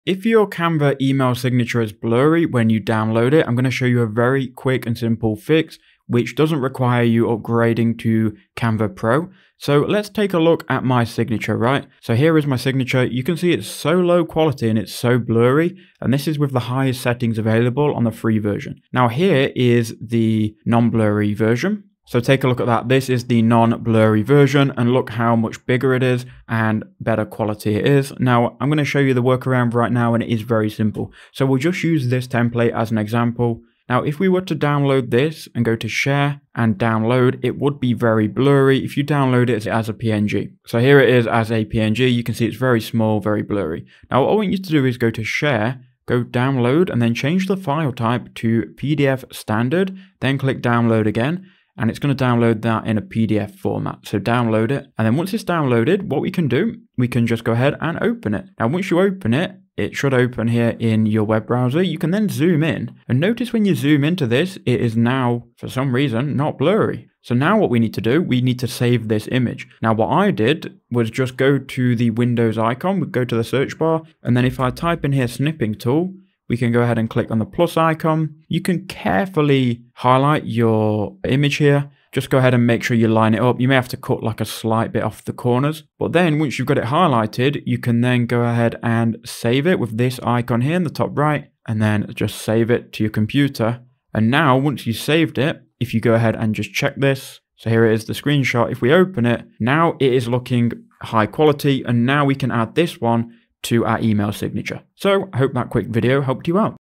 Canva Email Signature Blurry - Quick Fix
In this video, I show you Canva email Signature blurry quick fix. Did this video help? Thanks!